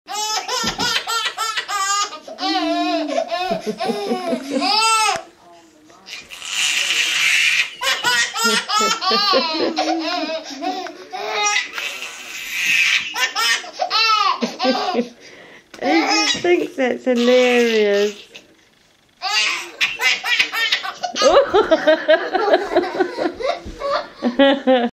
Laughing Baby Falls Over.
Big Brother making baby laugh so hard, he falls over.